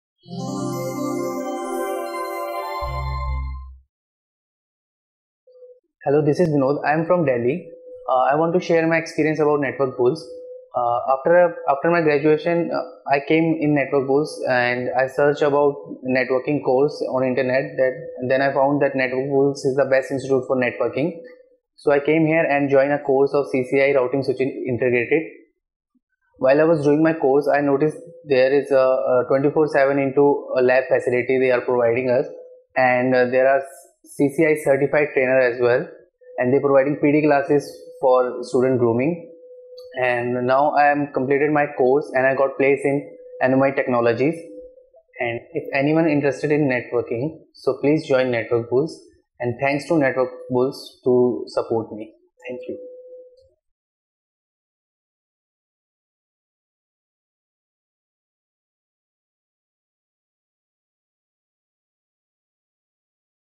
"I got job as Network Engineer after CCIE R&S Certification Course Training from NB" Vinod Review
Fresh graduates take Cisco certification training and fulfill their only career dream i.e to get a job in an IT company at a Network Engineer profile.

Recently Vinod from Delhi took CCIE R&S V5 training from NetworkBulls and made himself a spot at Anemoi technologies as a Network Engineer within a year after his graduation.

Do you wanna know how? Check Vinod’s testimonial and discover -

“Hello, Guys! This is Vinod Kumar from Delhi.

I always had a dream of having a career in the Networking field. Working in an IT organization as a Network Engineer, facing daily networking challenges and troubleshooting them on my own.

My drive to gain Networking training took a pace when I graduated and heard about the huge openings in the IT companies in the Network Engineer profile.

But I was clueless about Cisco certifications and that's what IT organizations were demanding in the applicants. Either a Cisco certification or a sufficient CCNA, CCNP, CCIE training would have worked for me but I had neither.

So, I decided to step in the Networking field with the help of Cisco certification courses training. When I searched for the best Cisco certification training institute in India over Google, I found numerous options claiming themselves to be the best one.

I had only one option left to find the best Cisco certification training institute for myself and it was visiting the listed institutes and compare them on my own.

I compared those institutes on various factors like Quality of education, lab facilities, trainers, reviews by enrolled students, etc. and I found Network Bulls the best.

I joined Network Bulls in CCIE R&S Integrated course training, learned CCNA R&S, CCNP R&S, and CCIE R&S by a majority of CCIE R&S certified trainers and CCIE R&S labs certified lab instructors.

Along with CCIE certification training, Network Bulls have regular PD and English speaking classes so that we could present ourselves in the best manner in front of the interviewer.

After CCNA, CCNP, CCIE R&S course completion, the dedicated placement cell at Network Bulls scheduled my interview with top IT companies and I got at Anemoi technologies.

I recommend every job seeker joining Network Bulls and take CCIE integrated course training. You will surely get a job if you put lots of hard work and dedication to your CCIE training.

Thank you, Network Bulls and cheers to an integrated program of NB.”

So, this was a success story of an ordinary graduate from Delhi, Mr. Vinod who got job placement in Anemoi technologies after CCIE R&S V5 Integrated course training from Network Bulls.

We admire his hard work, dedication, and passion for networking and feel glad to guide him.